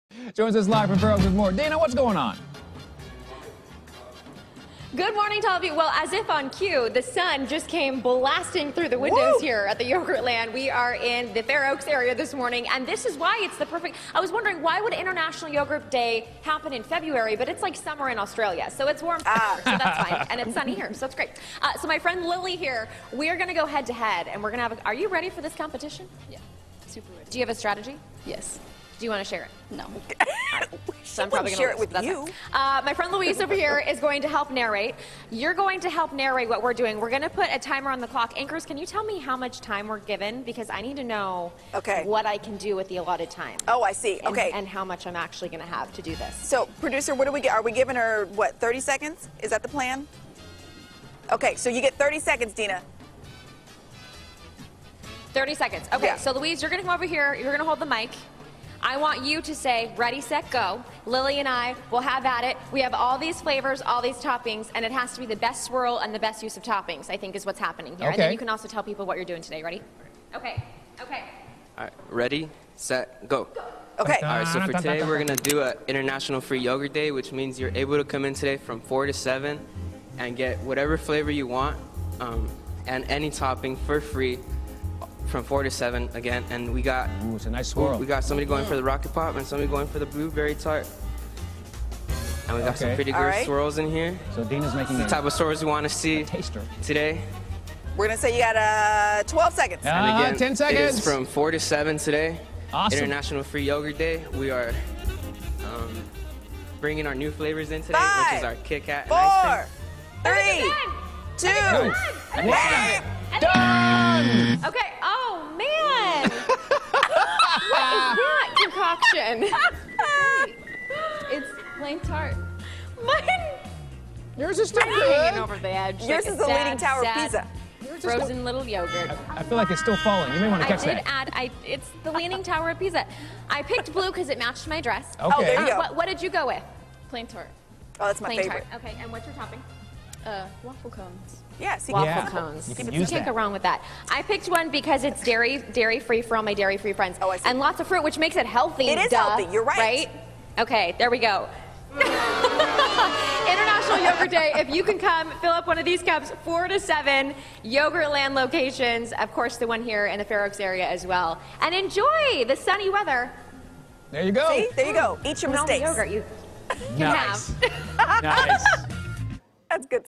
Froyo Games
Dina goes head-to-head with a yogurt land pro in an event we have dubbed "The Froyo Games."